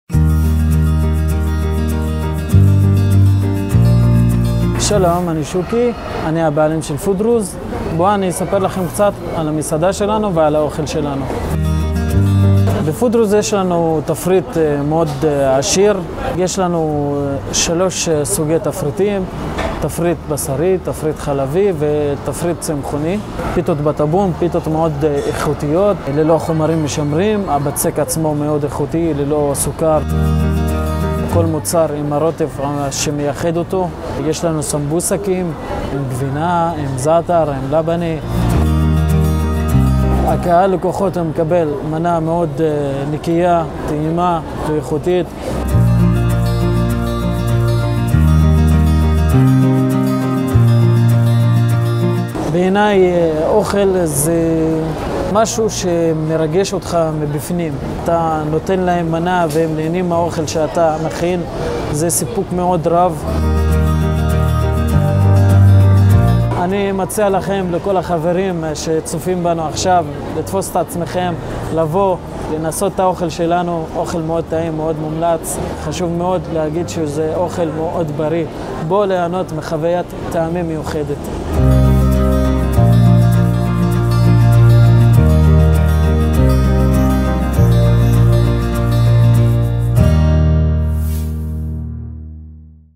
פוד רוז - FooDruze - אוכל דרוזי - ירכא - b144
פודרוז הינה מסעדה דרוזית המציעה תפריט עשיר באוכל דרוזי משובח ומגוון סלטים אותנטים מתוצרת הבית של פודרוז .בפודרוז על האיכות אין התפשרות כל המוצרים בתפריט הם מתוצרת הבית של פודרוז קיים סניף נוסף גוליס.
כל העסקים. כל האנשים. b144 כל המסעדות בצפון